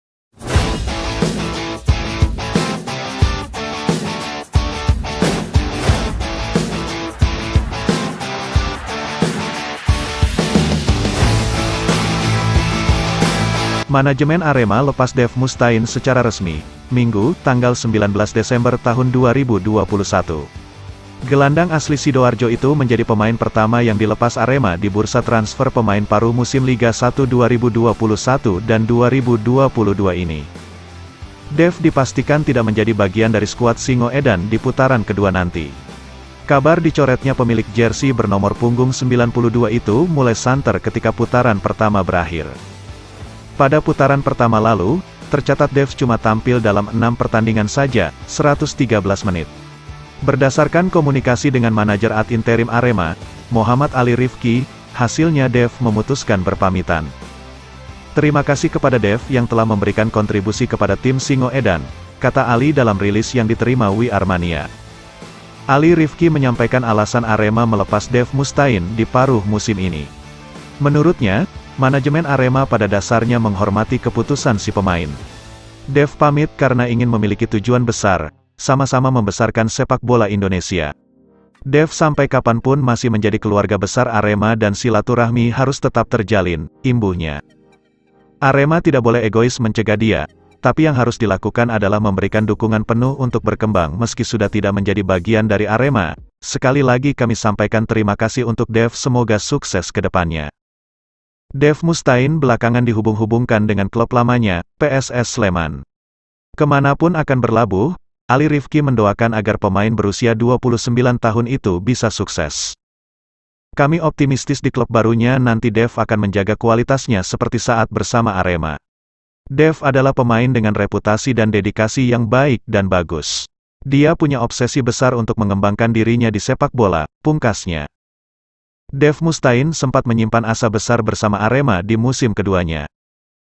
Berita Arema Hari ini, Arema FC RESMI Melepas Dave Mustaine...
Resmi Arema Lepas Dave Mustaine